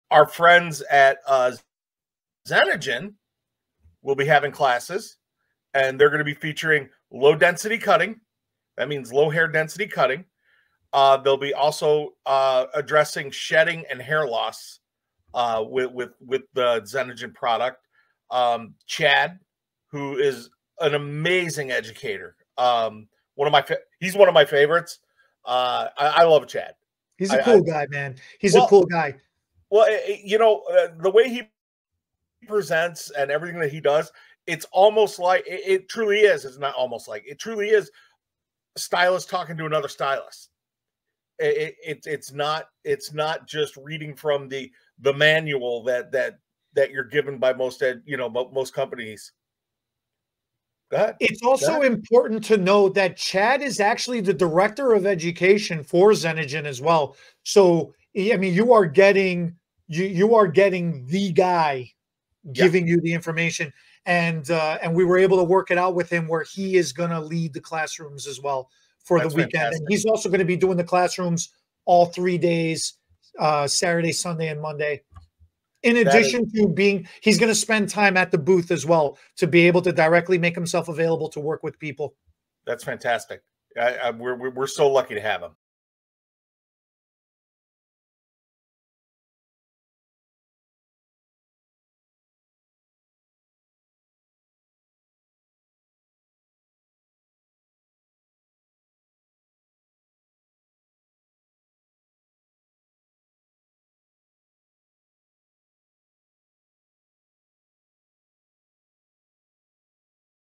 Hair Regrowth and Low Density Cutting Techniques FREE Education at America's Beauty Show 2024
Join us at America's Beauty Show 2024 for FREE education on hair regrowth and low density cutting techniques. Don't miss out on this valuable opportunity to learn and improve your skills in the beauty industry!

We are going to be at America's Beauty Show in Rosemont IL this upcoming weekend April 20-22.
Come visit us at Booth 1506 and Classroom 52 for free education on hair extensions, hair color toning, vegan smoothing products and low density hair cutting techniques!

Check out Hair's The Bling.
Unique Products. Innovative Solutions.